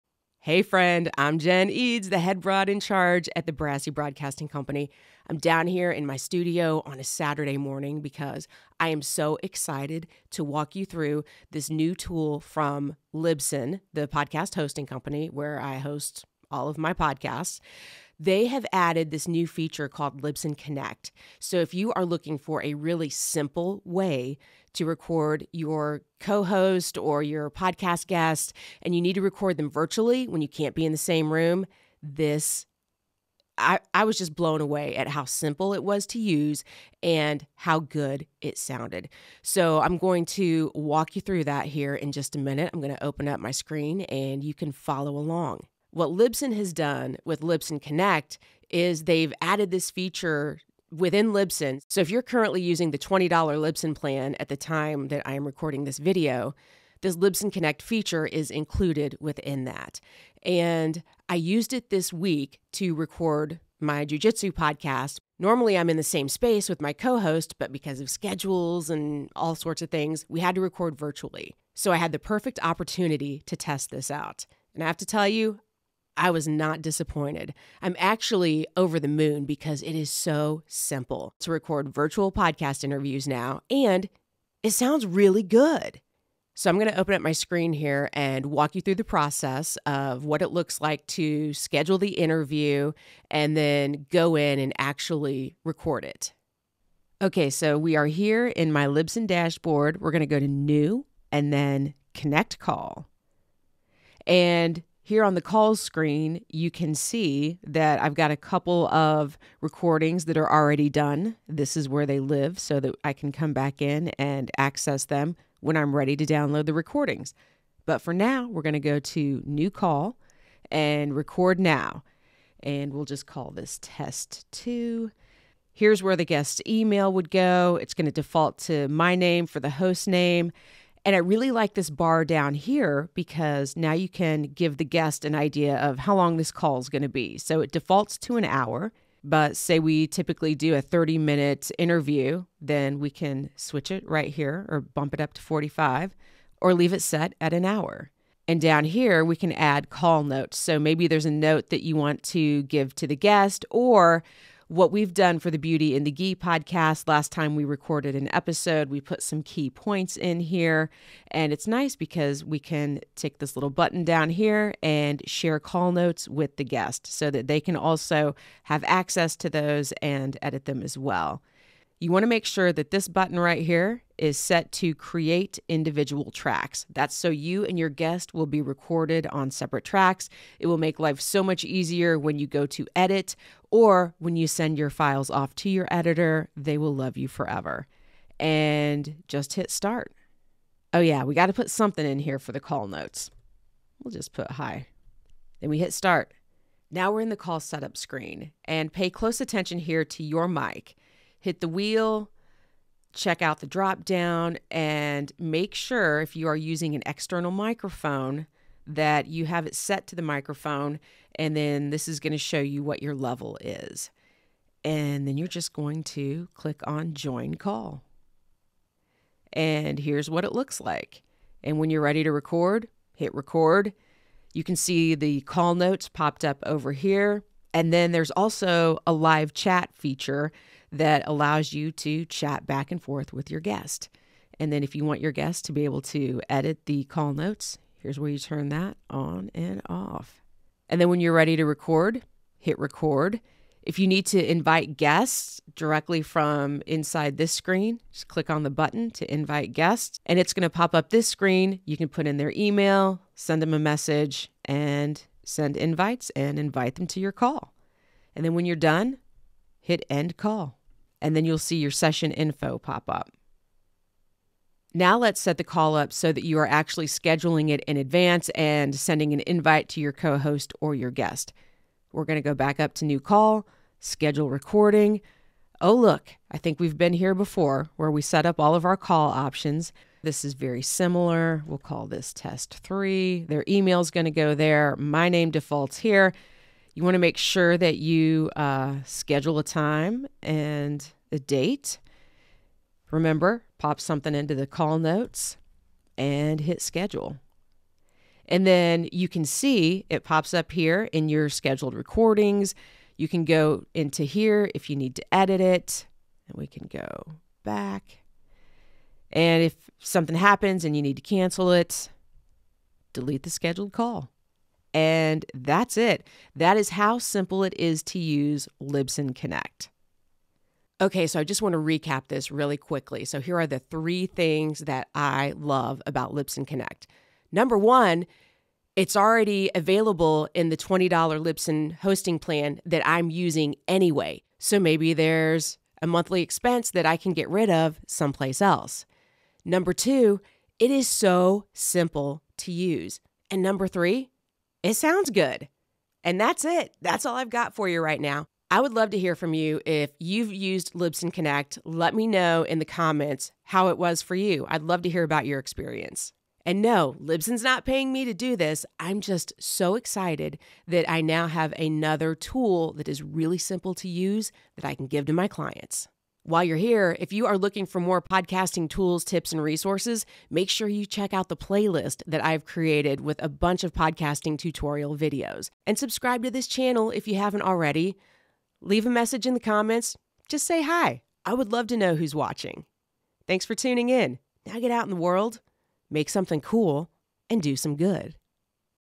Easy Podcast Guest Recording with Libsyn Connect | A Quickstart Guide
Libsyn Connect has made recording podcast guest interviews so much easier! I'm going to give you a quick tour of how to use Libsyn Connect to record your next remote podcast guest interview.

This is a simple way to get a professional sounding recording. It's easy for both you and your guests. You can schedule interviews in advance and send the calendar invite to your guest or co-host.

Check out this podcast to listen to the audio quality. It was recorded using Libsyn Connect.

Let’s Connect

2021 MacBook Pro
Microphone: Shure MV7
Shure 115
Video made with Camtasia
Video recorded with Canon EOSRP